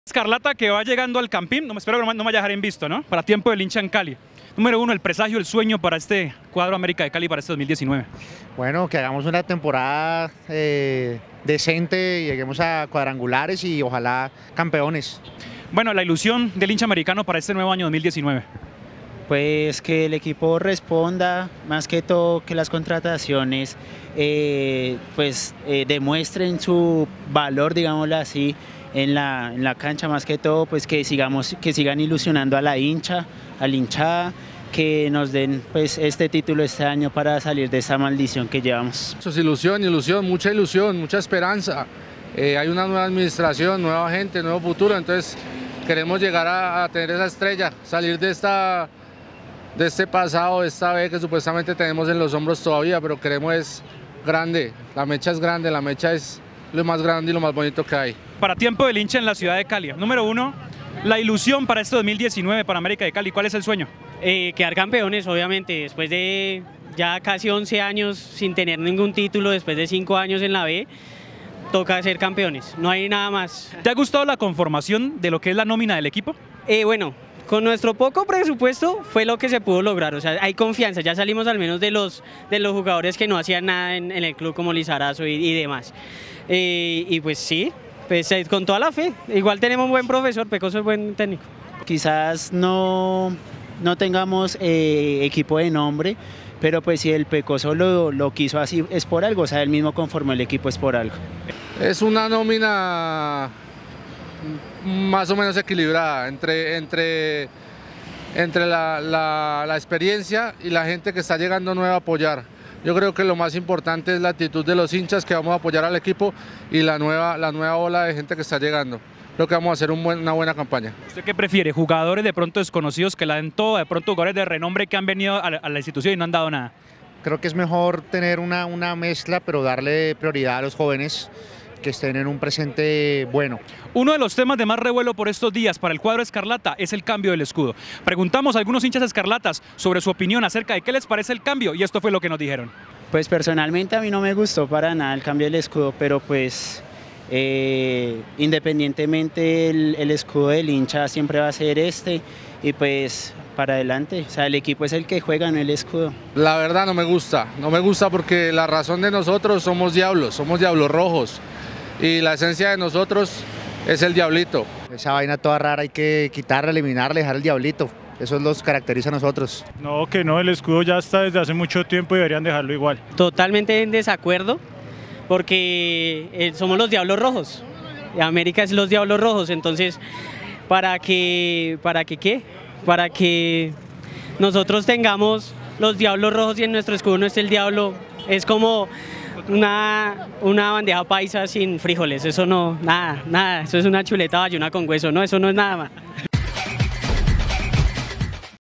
2019: ¿LE CREE LA HINCHADA AL ROJO?
Hablamos con la hinchada escarlata en la previa al juego con Nacional, su presagio para el 2019 con el equipo y el tema de la modificación del escudo entre lo mas relevante.